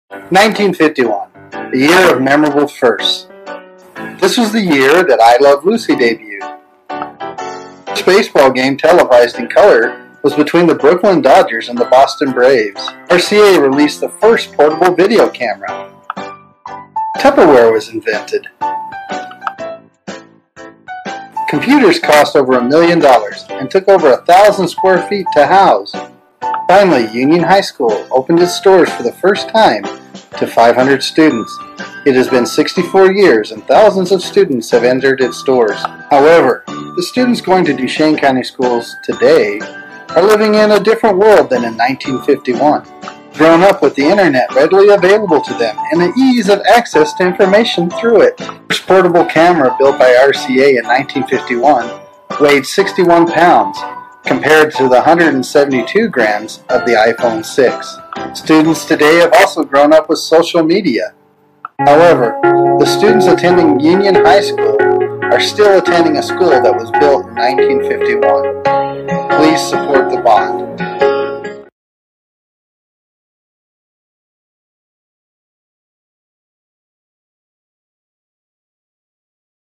Bond Video 2
This is a short video talking about the changes between 1951 and today. I hope you enjoy this video. Please remember to support the bond.